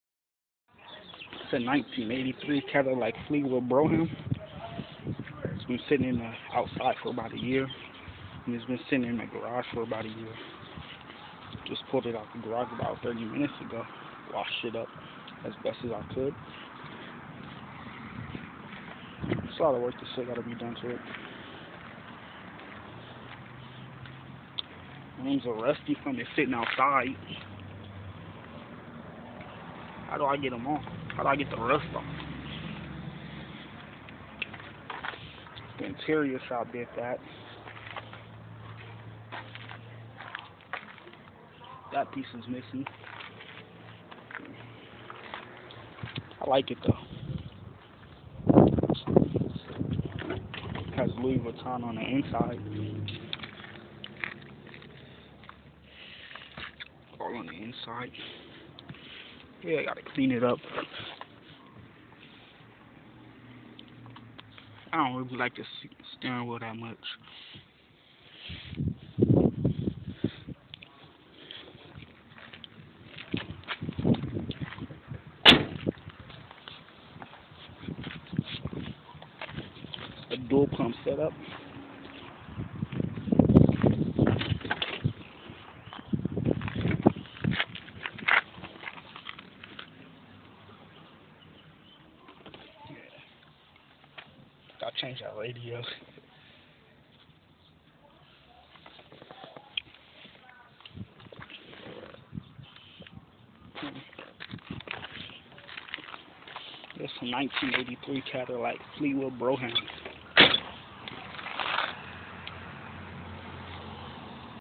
1983 Cadillac Fleetwood Brougham Lowrider
It has been sitting up for a long time.